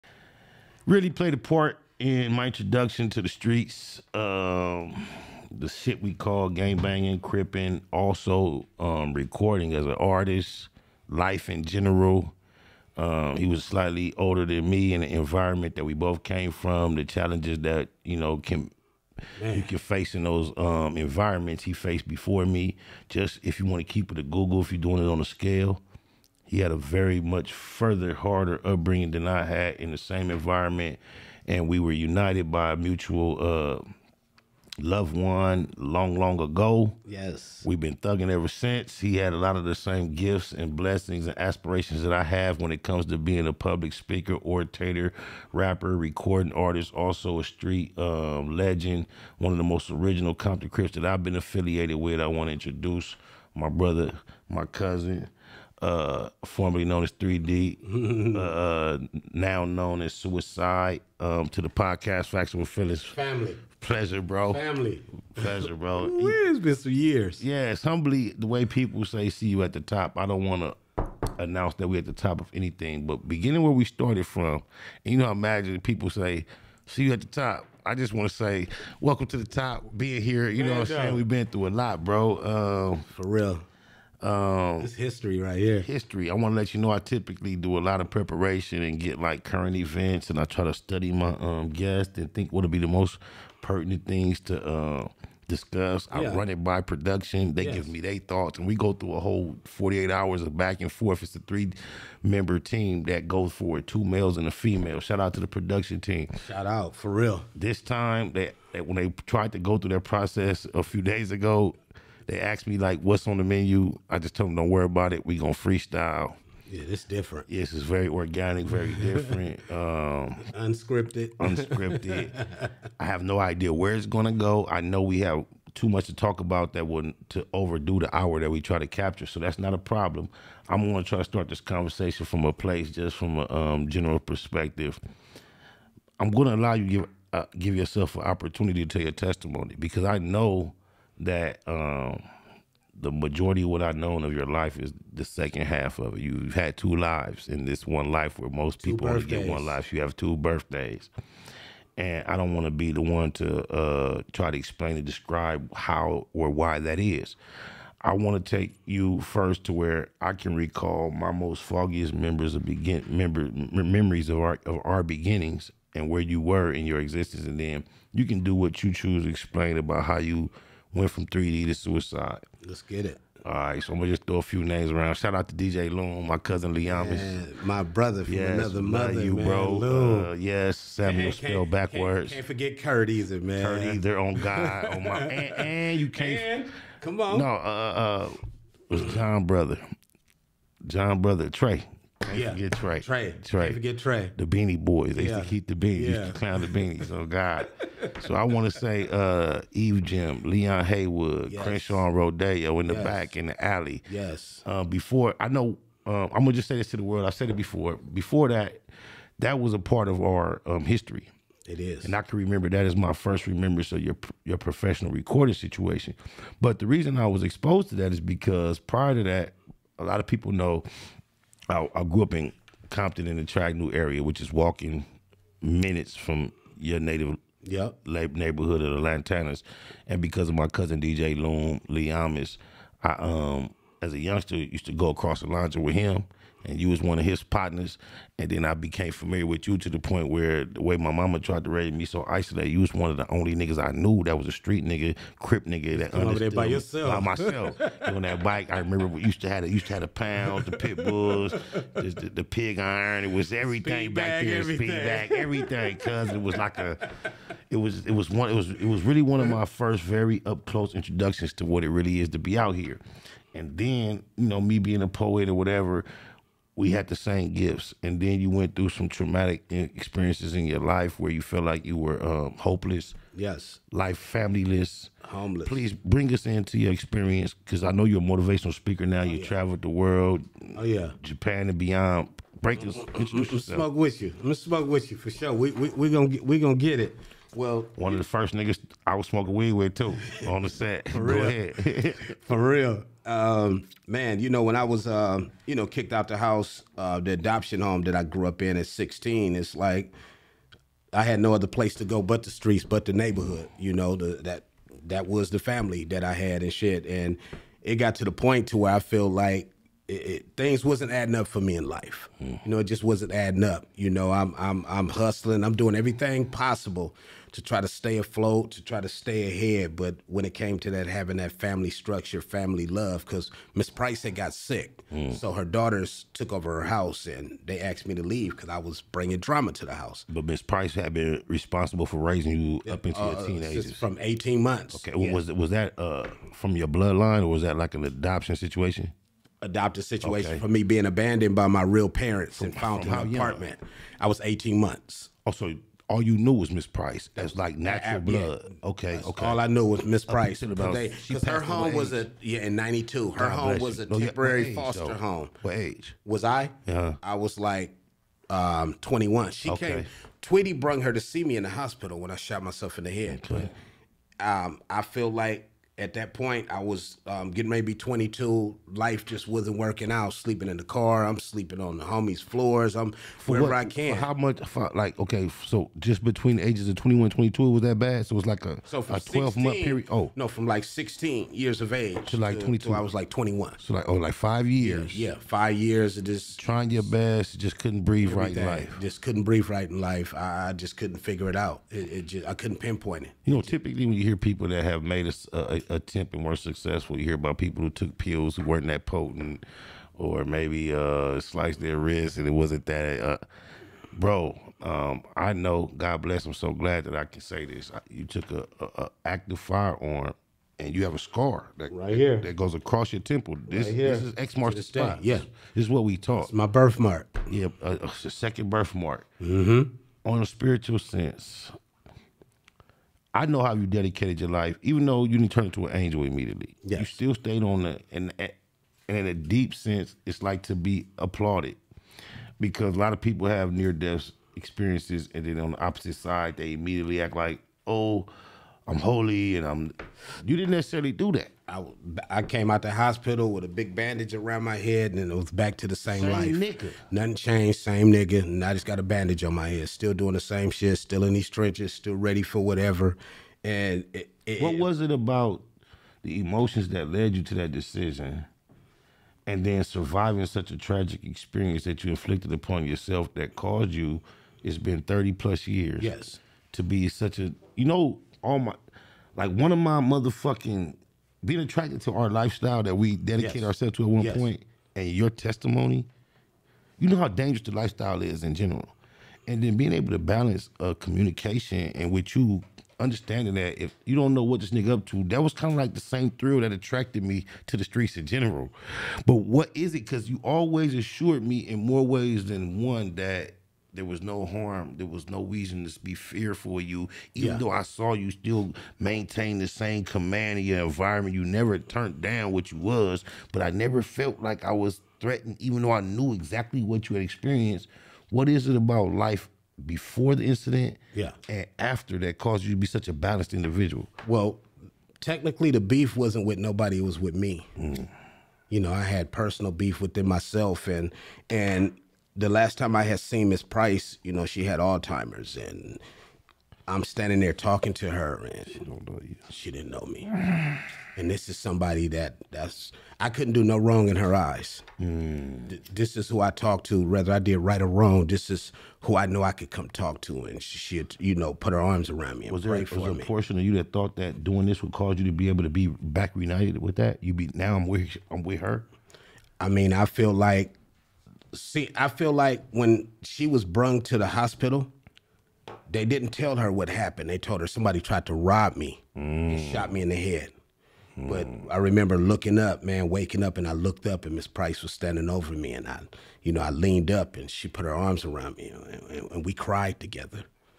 OG Cuicide from Lantana Blocc growing up in Compton as adopted kid (FOF17)
In this Episode 17 of the Factz Ova Feelinz podcast with Spider Loc, he is joined with guest Cuicide from Lantana Blocc Compton Crips discusses growing up in Compton by his adopted mom.